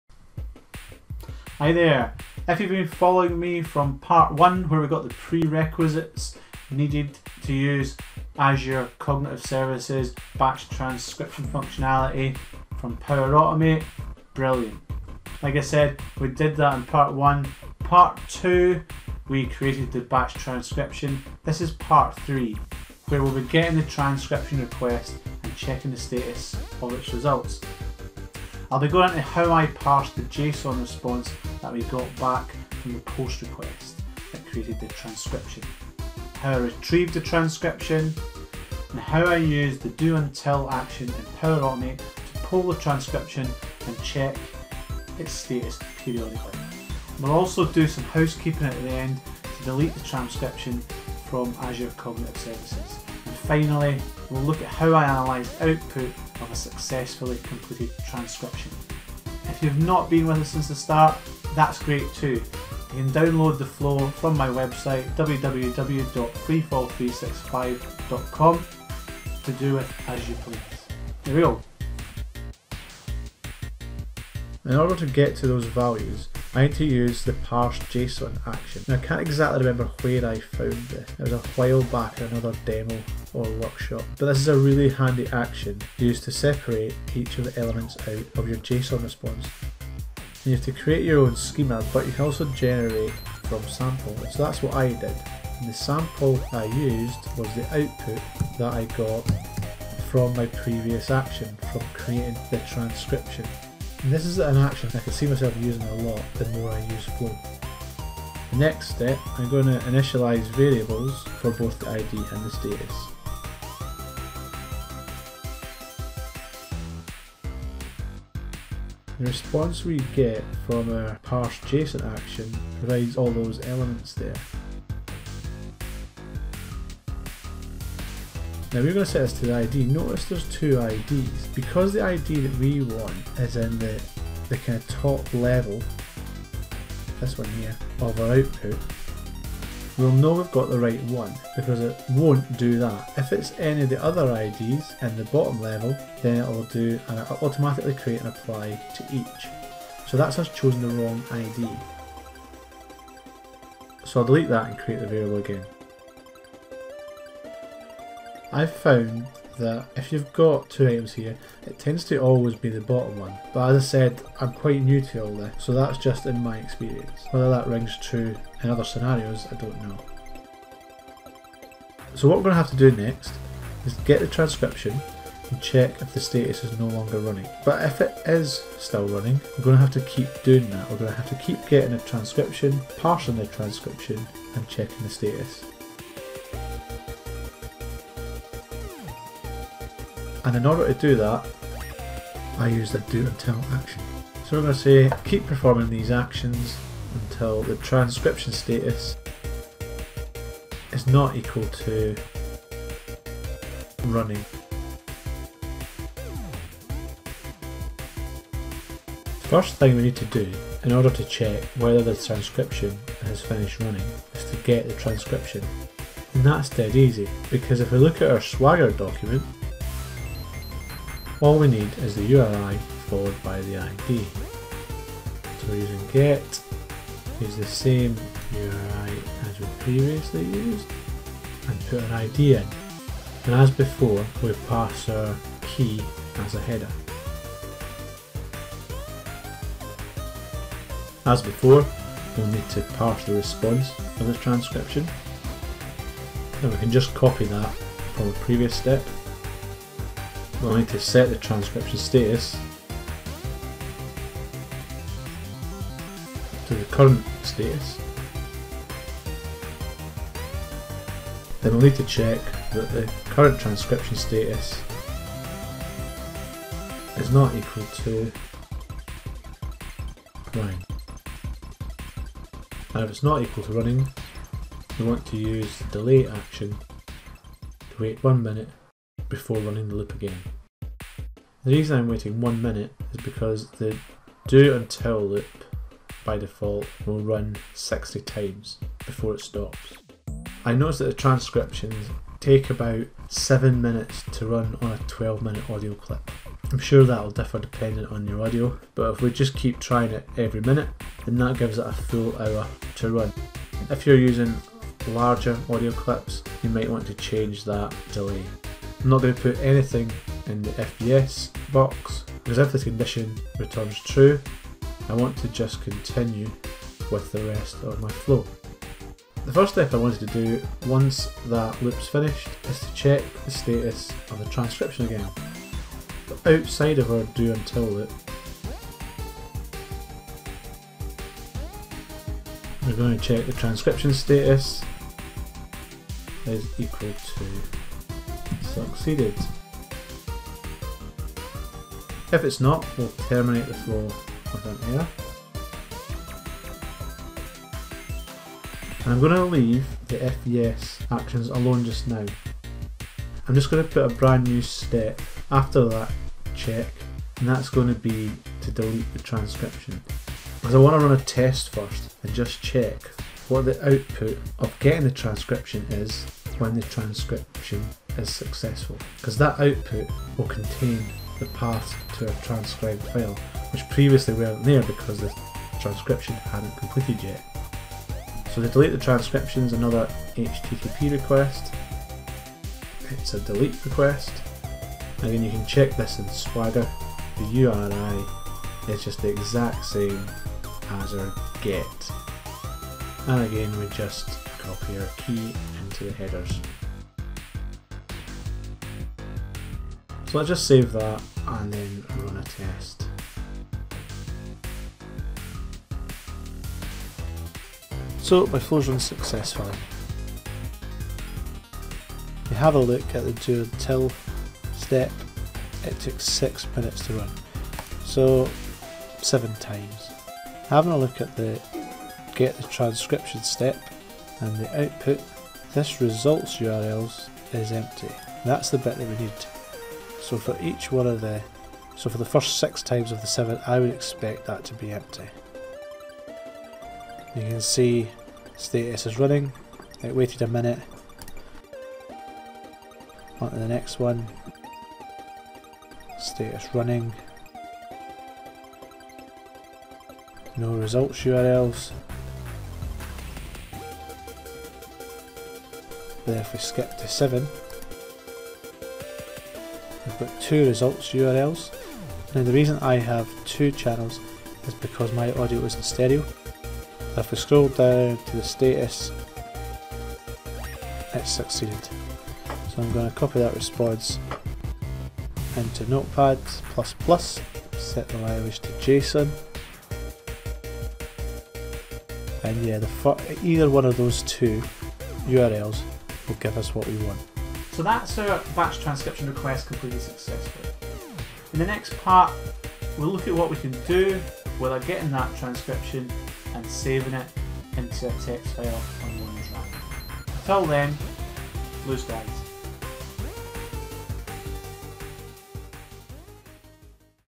Checking the Transcription Status: Azure Cognitive Services Batch Transcription with Power Automate
This is video three in a four video series where I demonstrate how to use Power Automate to transcribe audio files using Azure Cognitive Services Batch Transcription feature and my thought process through learning about the steps involved.

Part 3: Retrieving the Transcription request, checking the status and results

In this part I summarise -

 - How I parsed the JSON response that we got back from the POST request.
 - How to retrieve the transcription
 - How I used the Do Until action in Power Automate to poll the transcription and check it's status periodically
 - I did some housekeeping and deleted the Transcription from Azure Cognitive Services.
 - And finally how I analysed the output of a completed transcription request.